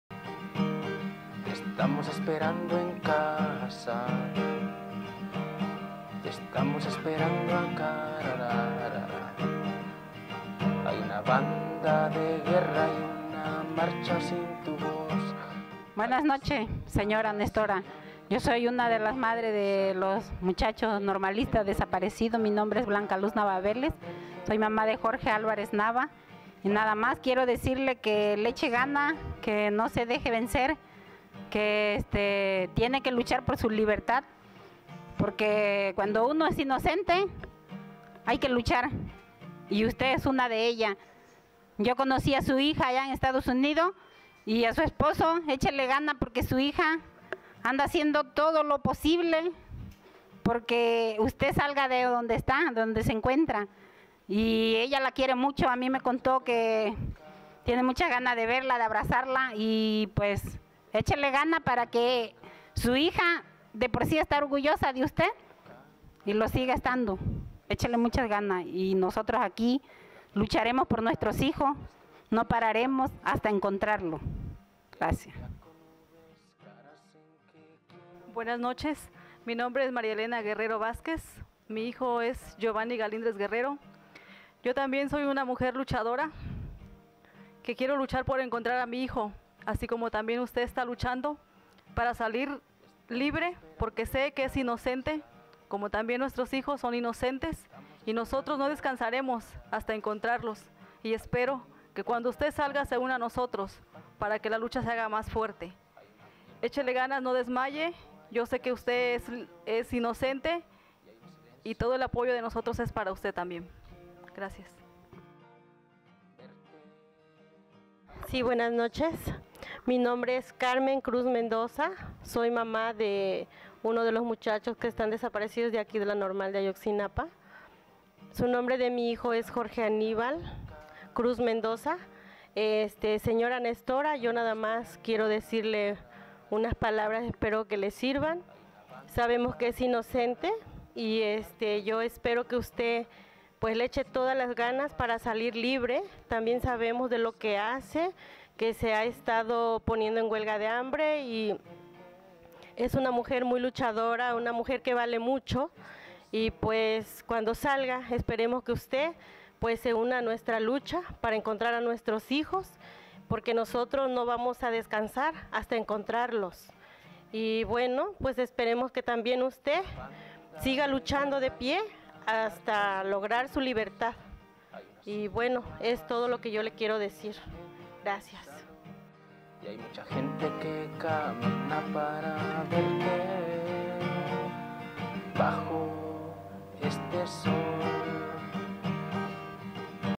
Madres de normalistas desaparecidos envían mensaje a Nestora Salgado
“Nestora, yo también soy una mujer luchadora, siga luchando de pie para lograr su libertad”
- Madres de normalistas desaparecidos